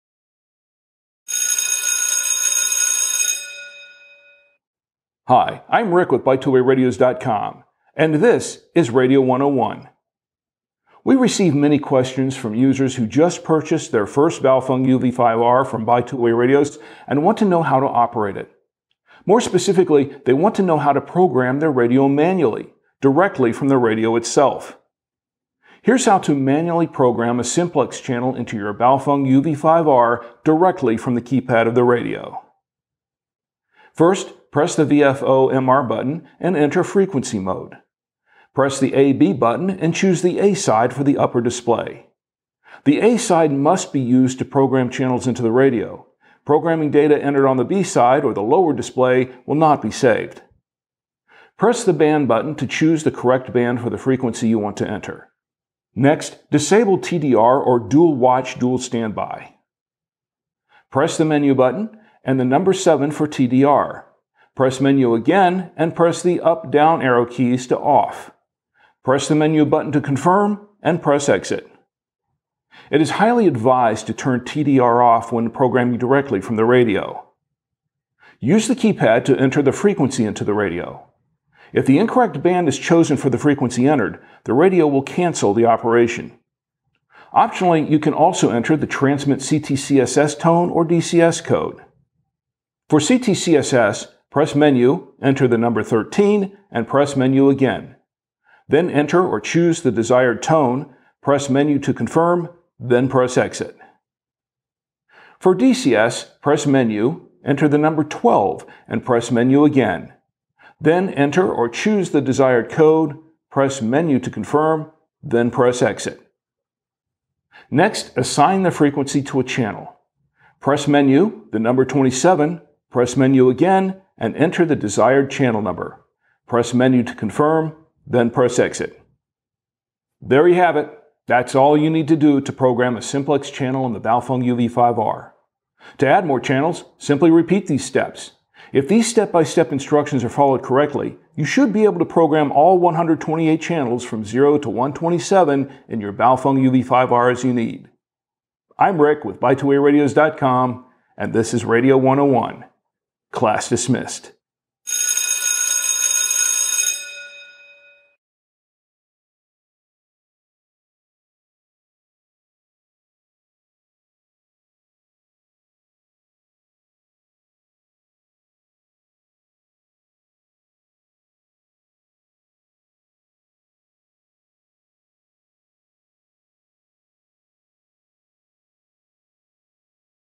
How To Program the Baofeng UV-5R From the Keypad | Radio 101
It's one of the most frequently asked questions we get about one of the most popular radios in the world. How do you program the Baofeng UV-5R dual band two way radio directly from the radio itself? In this episode of Radio 101, we show you. It's not as difficult as you think. The Baofeng UV-5R Dual Band UHF/VHF Two Way Radio is available